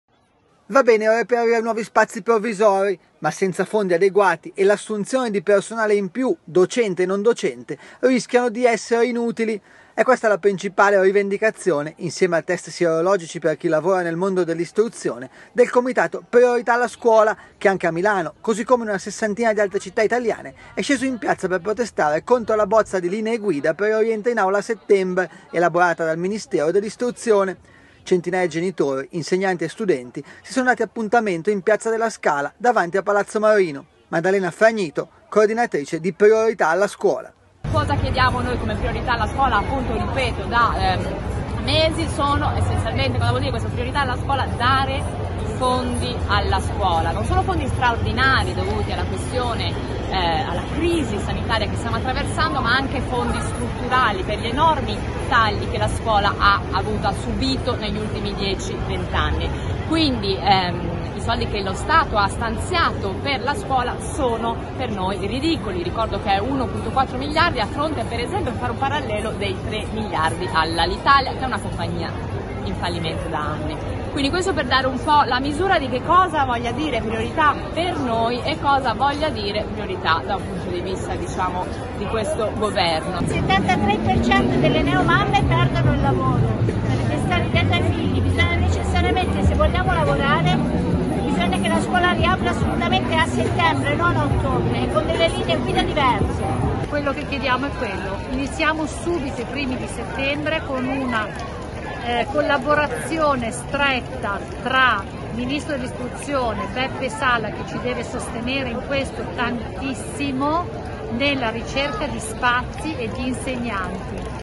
Sessanta città in piazza per la scuola e contro le linee guida
Milano, 25 giu. (askanews) - Va bene reperire nuovi spazi provvisori, ma senza fondi adeguati e l'assunzione di personale in più, docente e non docente, rischiano di essere inutili. È questa la principale di rivendicazione, insieme a test sierologici per chi lavora nel mondo dell'istruzione, del comitato Priorità alla scuola che anche a Milano, così come in una sessantina di città italiane, è sceso in piazza per protestare contra la bozza di linee guida per il rientro in aula a settembre elaborata dal ministero dell'Istruzione, coordinato da Maddalena Frangito. Centinaia di genitori, insegnanti e studenti che si sono dati appuntamento in piazza della Scala, davanti a Palazzo Marino.